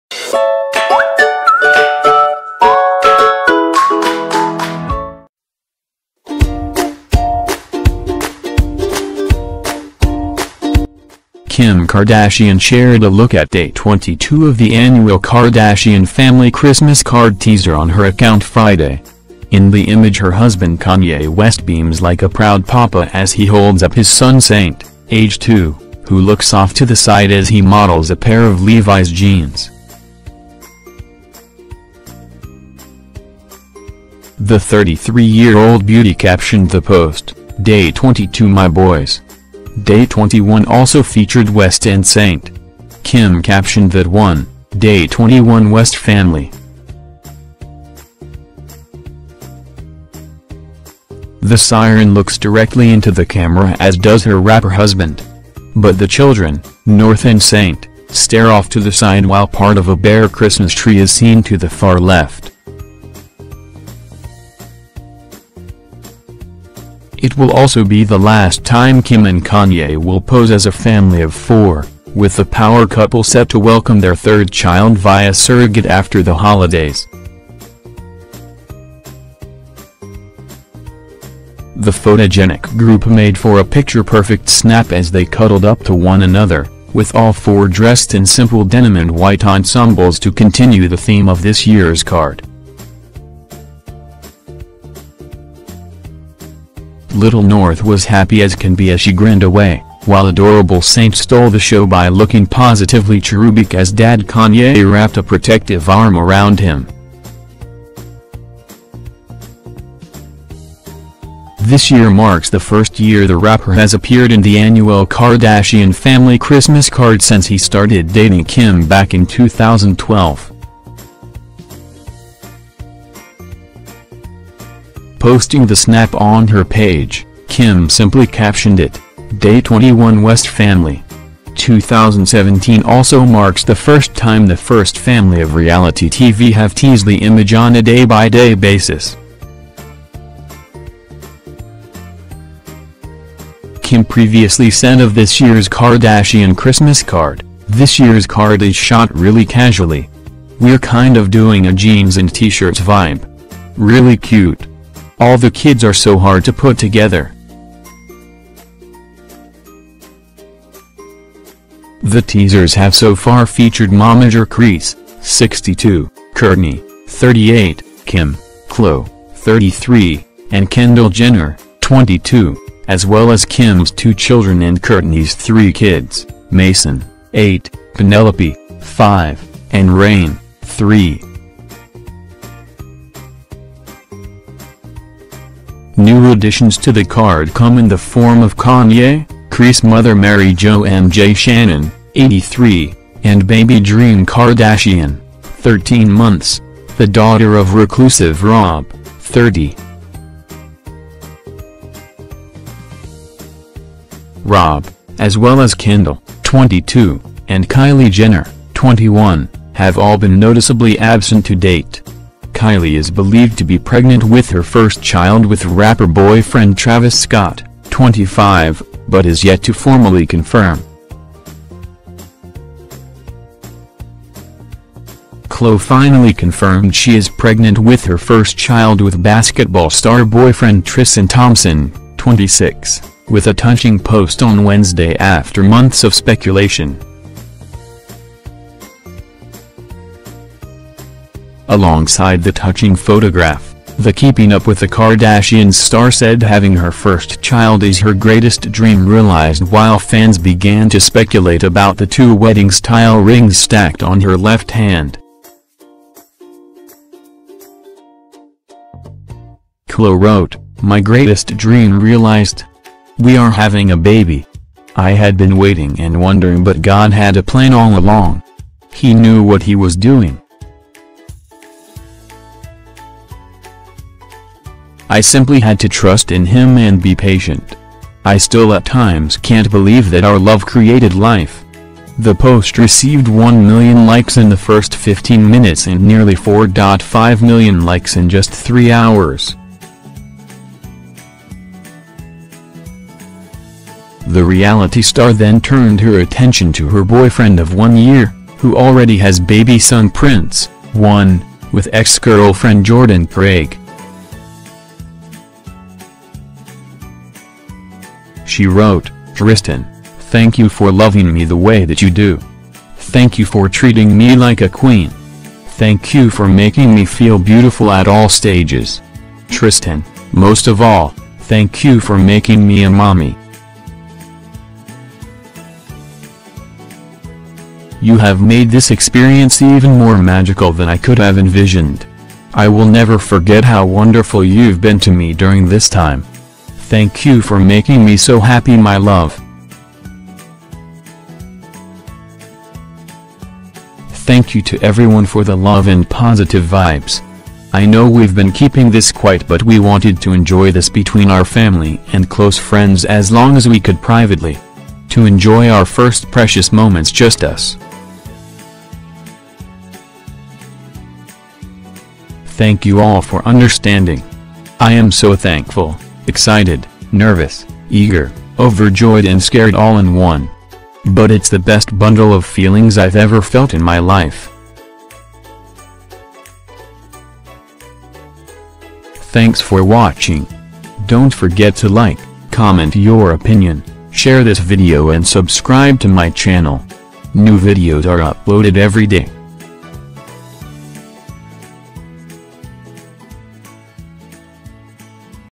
Kim Kardashian shares Kanye then Saint Christmas photograph toughness Kim Kardashian Breaking News
Kim Kardashian shared a look at day 22 of the annual Kardashian family Christmas card teaser on her Instagram account Friday. In the image her husband Kanye West beams like a proud papa as he holds up his son Saint aged two who looks off to the side as he models a pair of Levis jeans.
Demi Lovato and Charlie Hunnam revel in late lunch longevity They became buddies sinc Breaking News: